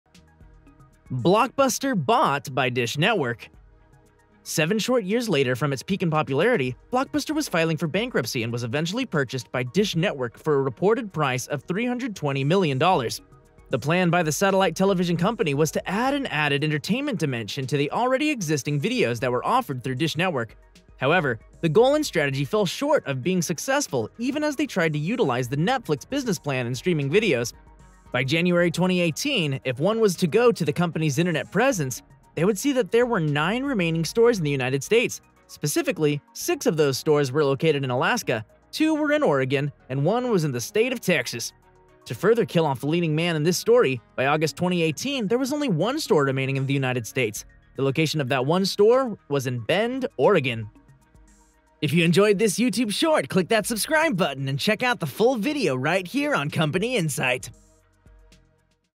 Rise and Fall of Blockbuster Part 4 : Blockbuster Bought by Dish network and Bankruptcy
Blockbuster Bought by Dish Network

Seven short years later from its peak in popularity, Blockbuster was filing for bankruptcy and was eventually purchased by Dish Network for a reported price of $320 million. The plan by the satellite television company was to add an added entertainment dimension to the already existing videos that were offered through Dish Network.

However, the goal and strategy fell short of being successful even as they tried to utilize the Netflix business plan in streaming videos. Looking back, it’s really incredible to think that Blockbuster could’ve beat Netflix at their own game.

By January 2018 if one was to go to the company's Internet presence, they would see that there were nine remaining stores in the United States. Specifically, six of those stores were located in Alaska, two were in Oregon and one was in the state of Texas. To further kill off the leading man in this movie, by August 2018 there was only one store remaining in the United States. The location of that store was in Bend, Oregon.